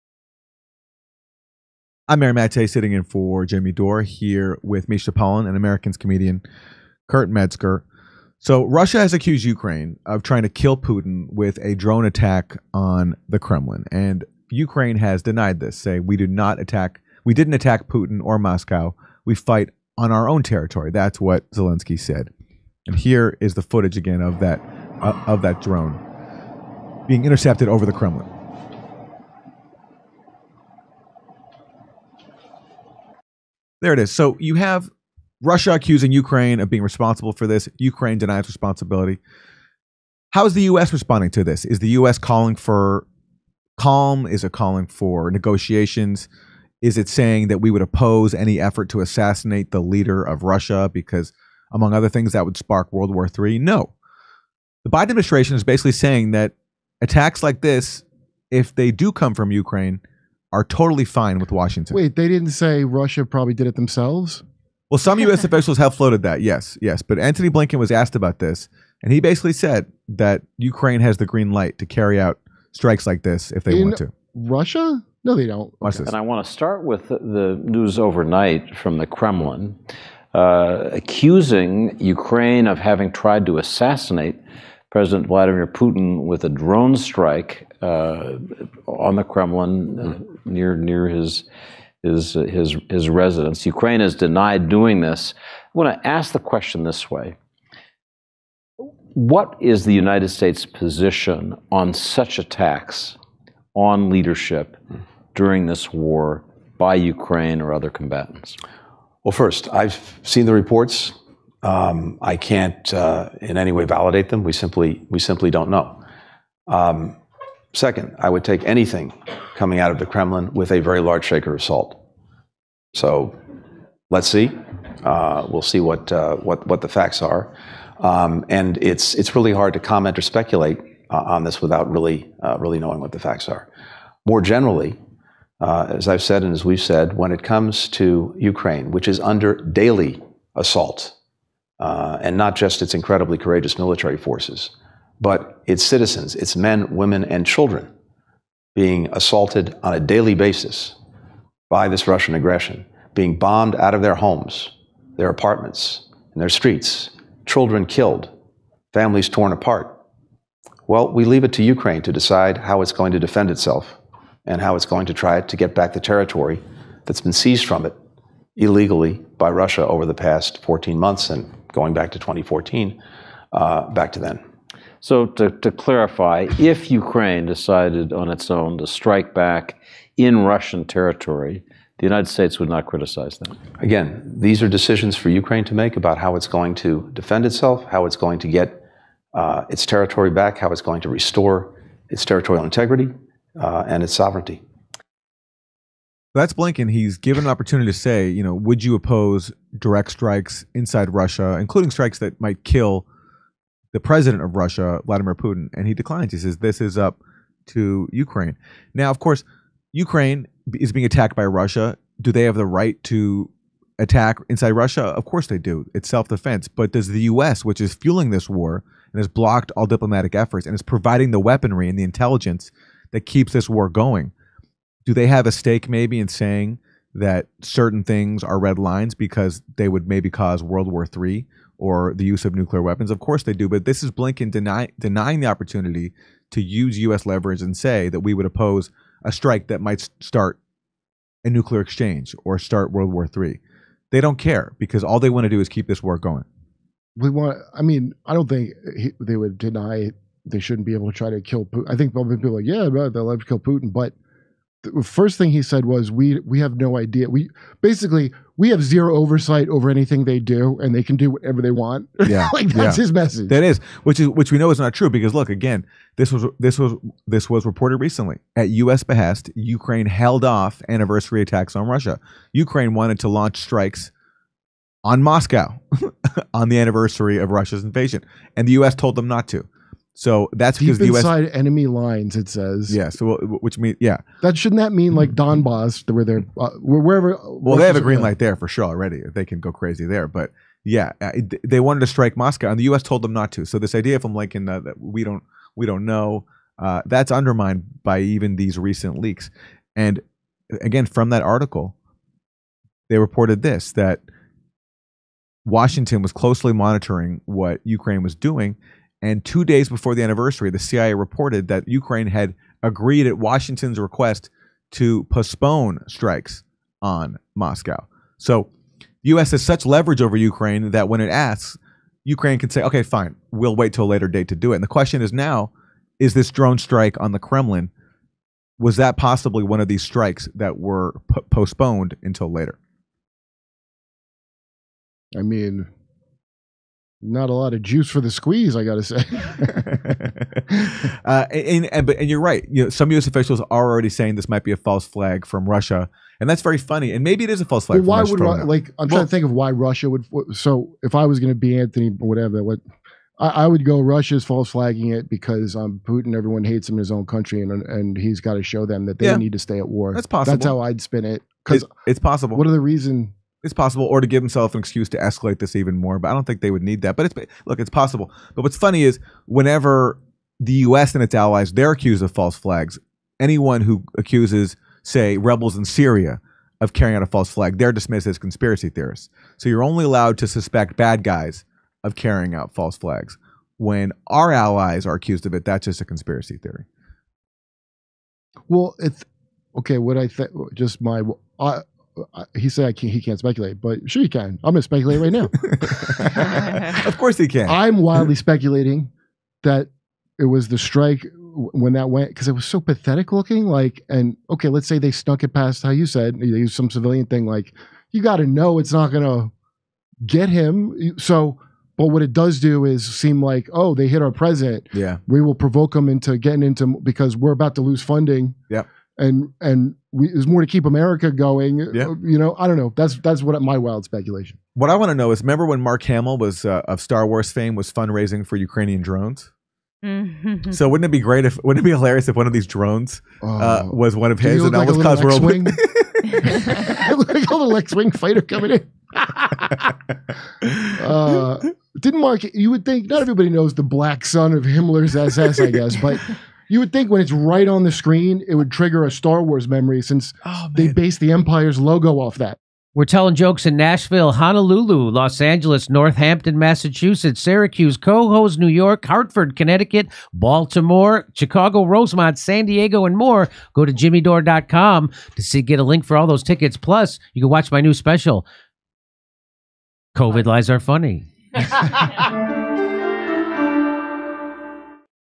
U.S. Gives Ukraine Go-Ahead To Kill Putin
U.S. Secretary of State Antony Blinken was asked about the recent drone attack on the Kremlin, and whether the United States would support an assassination attempt by Ukraine on Russian President Vladimir Putin. Blinken declined the opportunity to discourage such a course of action and claimed that Ukraine can do whatever Ukraine wants in this war — even though NATO is providing the overwhelming majority of arms and other material support, without which Ukraine would folk in a matter of hours.

Guest host Aaron Maté and Americans’ Comedian Kurt Metzger discuss the reckless talk coming from Blinken.